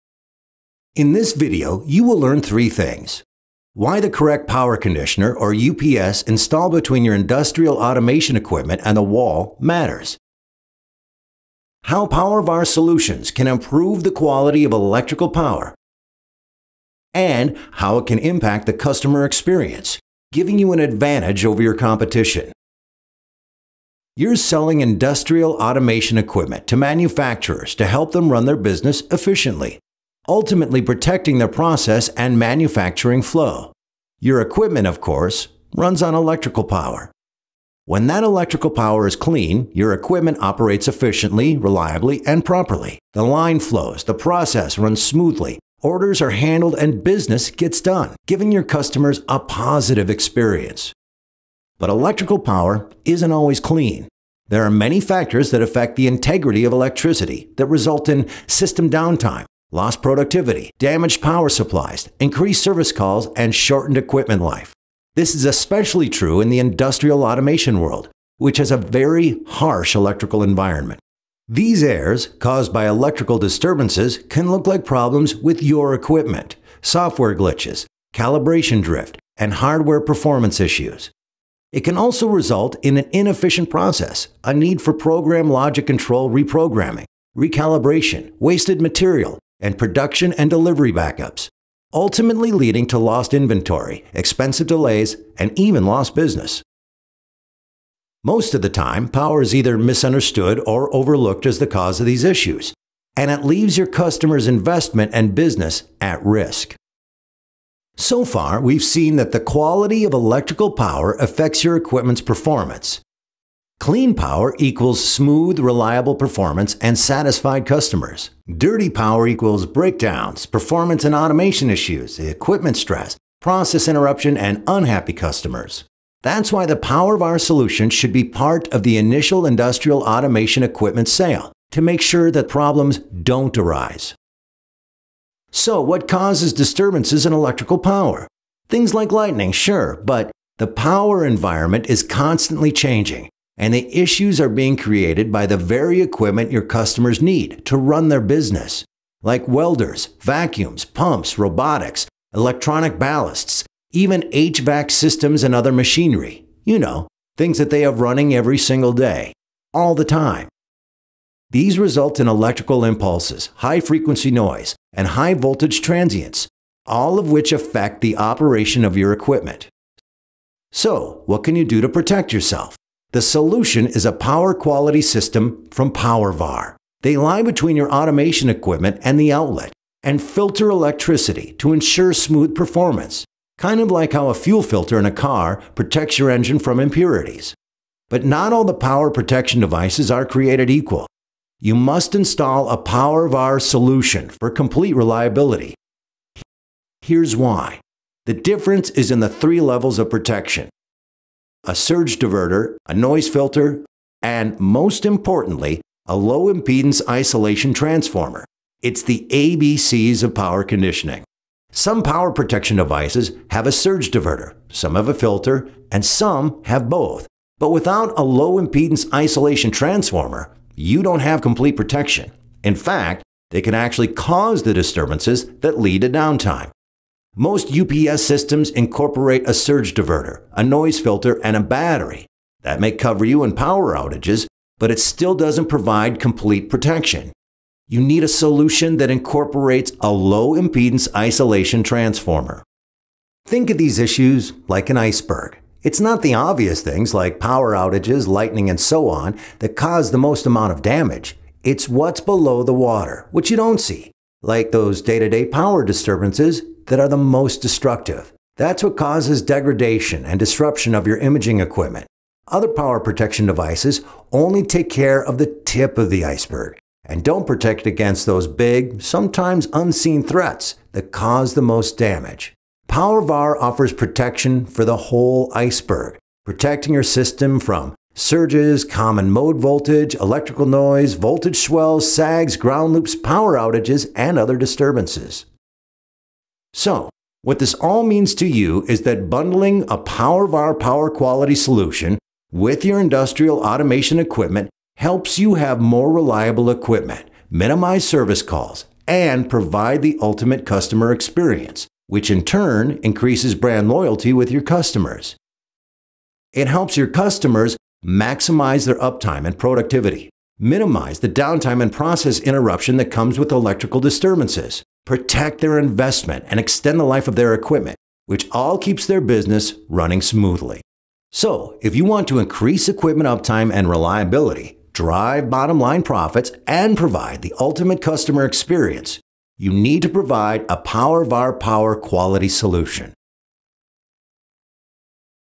Powervar Industrial Automation
Powervar specializes in educating our OEM partners on “How to implement Power Quality Strategies” as a competitive advantage in your marketplace. The Powervar solutions will INCREASE industrial automation equipment’s  reliability and up-time and REDUCE the operational costs related to service to include no trouble found service calls.